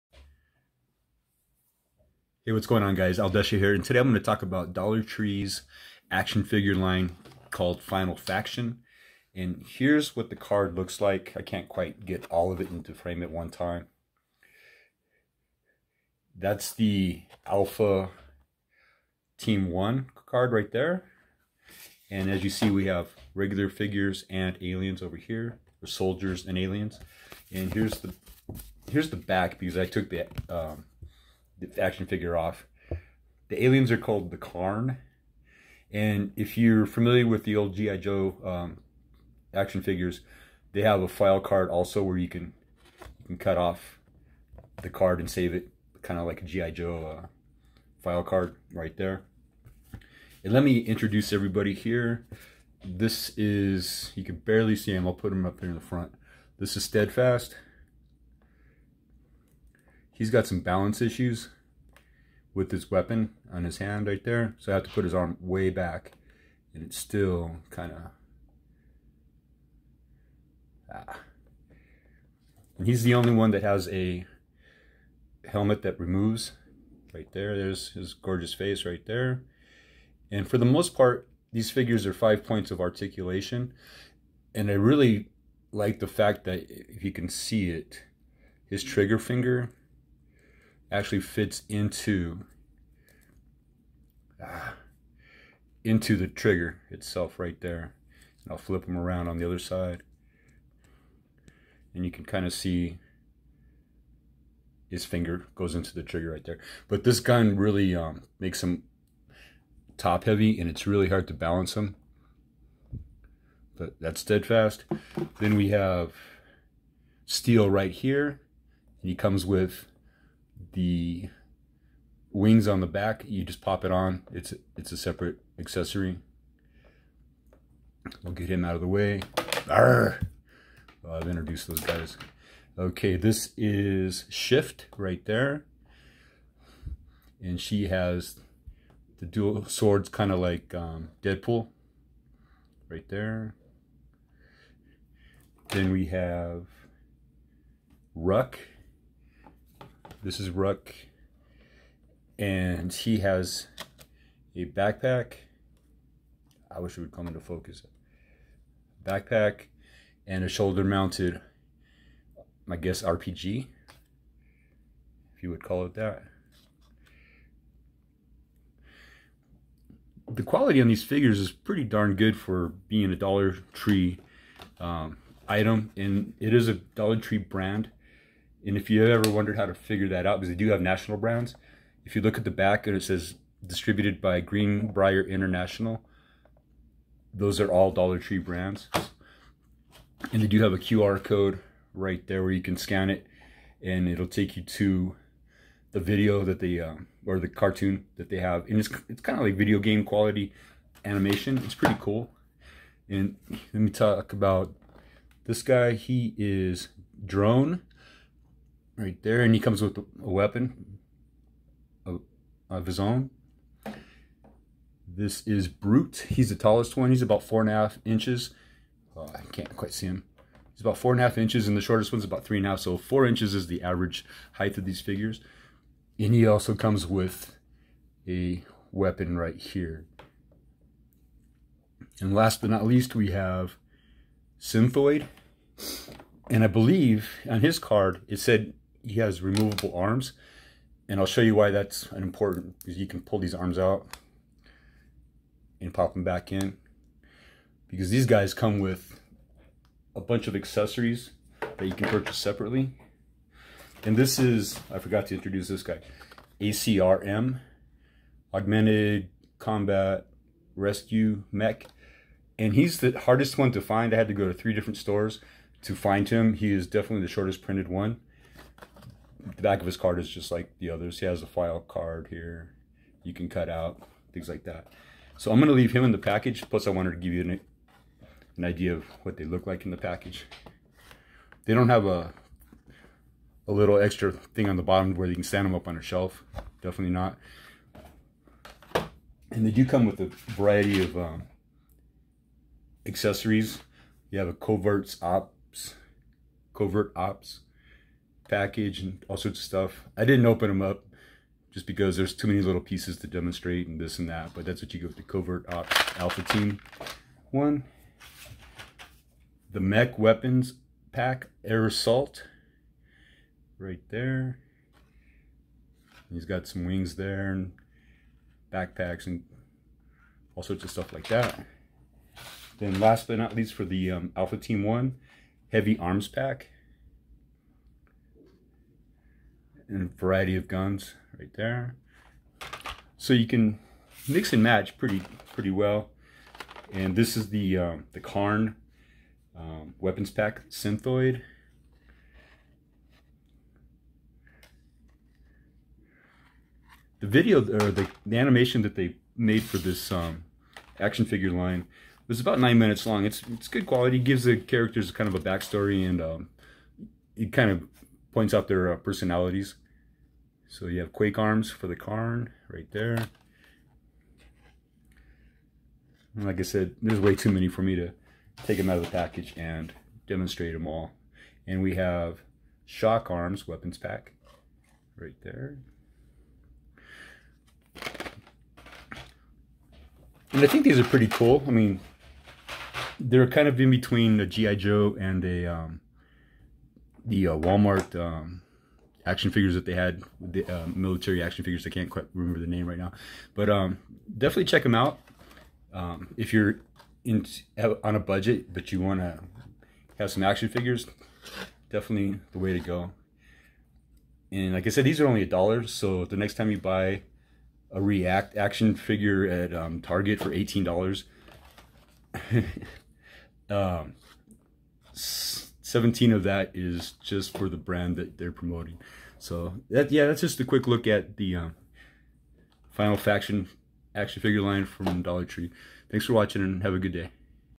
Dollar Tree Final Faction Action Figures!
Today I talk about Dollar Tree's Final Faction action figures.